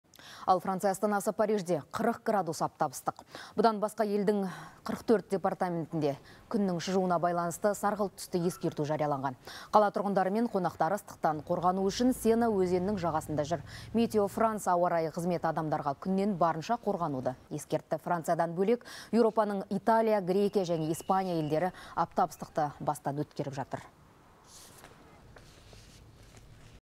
Францияда аптап ыстыққа байланысты дабыл қағылды
Франция астанасы Парижде 40 градус аптап ыстық. Сонымен қатар елдің 44 департаментінде күннің шыжуына байланысты, сарғылт түсті ескерту жарияланған. Қала тұрғындары мен қонақтары ыстықтан қорғану үшін Сена өзенінің жағасында жүр. «Метео Франс» ауа-райы қызметі халыққа күннен барынша қорғануды ескертті. Франциядан бөлек Еуропаның Италия, Грекия және Испания елдері аптап ыстықты бастан өткеріп жатыр.